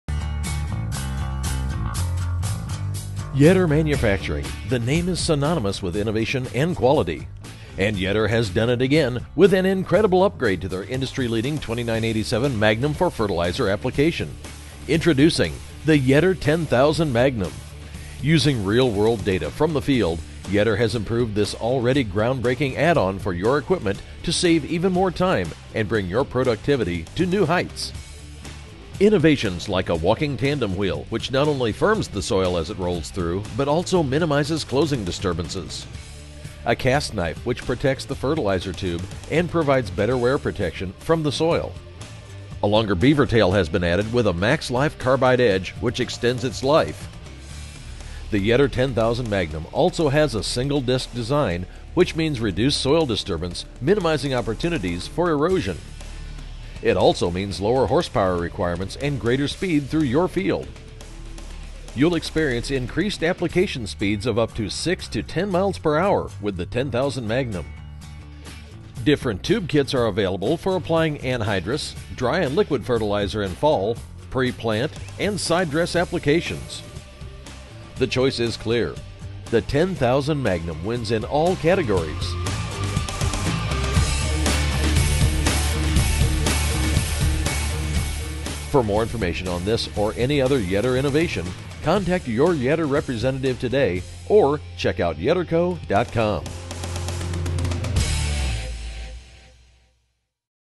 Get to Know the Yetter 10,000 Mangnum™ Opener for Fertilizer Application
The 10,000 Magnum Opener is a versatile fertilizer coulter for fall, pre-plant, and side-dress application of dry, liquid, and anhydrous fertilizers. Operating at speeds of up to 12 mph with minimal disturbance, the Magnum saves time and input costs.

10,000 Magnum Features:
• Offers tube kit options for applying anhydrous, liquid, or dry fertilizer at high application speeds
• Ideal for fall, pre-plant, or side-dress application
• Mounting bracket available for optional residue manager to create a combo unit that clears strips with minimal soil disturbance
• Single-disc design cuts through residue with minimal soil disturbance
• Opener has a 22.6" blade set at a 4º angle
• Walking tandem wheel option firms soil as it rolls through the field behind the SharkTooth® wheel and Magnum coulter
• Closing wheels utilize three down pressure settings
• Spring-loaded knife with carbide insert extends wear and life between replacements
• Quick-to-replace beaver tails on the cast knife utilize MaxLife Carbide Edge™ weld to extend life between replacements